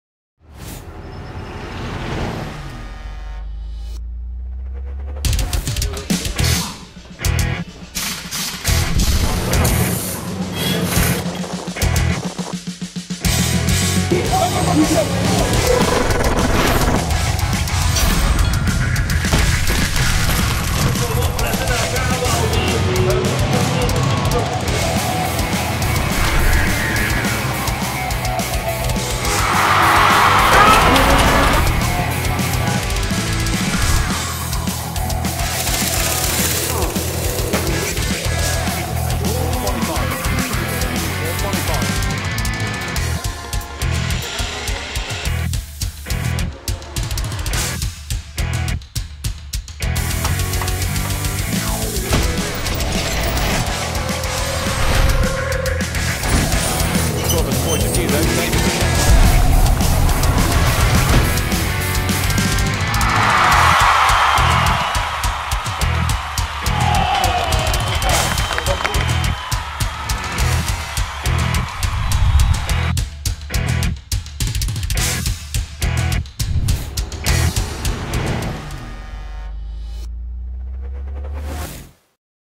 Taking the race truck to the limit and getting it back in place again
Changing an engine in 1 hour? Exchanging clutches and gear boxes last minute? Pressure is on. Welcome to the everyday life of the teams‘ mechanics. Especially in truck racing – the probably purest form of racing – there is lots of contact throughout the races, leaving a lot of work to the teams‘ mechanics. It’s not only about figuring out the perfect set-up for the truck. The truck racers take the trucks to their limits in each session. A stress test for the technical parts - and the mechanics. The schedule is tight. 2 free training sessions, 2 qualifyings, 2 super poles, 4 truck races within 2 days doesn’t leave much time to get everything back in place or solve technical problems. The truck racer is only as good as his team. Winning races is literally the result of strong, committed team work.